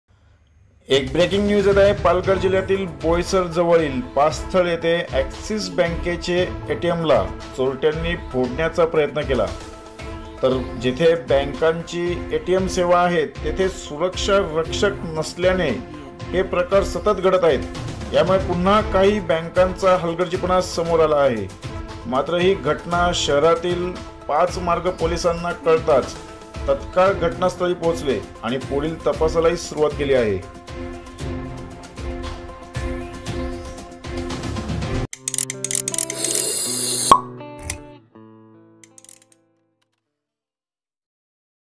AXIS BANK ATM चोरट्यांनी केला फोडण्याचा प्रयत्न॥ बोईसर॥पास्थळ
बोईसर जवळील पास्थळ येथे ऍक्सिस बँकेचे एटीएमला चोरट्यानी फोडण्याचा प्रयत्न केला ,मात्र जेथे बँकांची एटीएम सेवा आहेत तेथे सुरक्षा रक्षक नसल्याने हे प्रकार सतत घडत आहेत यामुळे पुन्हा काहि बँकांचा हलगर्जीपणा समोर आला आहे मात्र हि घटना शहरातिल
पाचमार्ग पोलीसांना कळताच तत्काळ घटनास्थळी पोहोचले आणी पुढिल तपासाला सुरवातही केली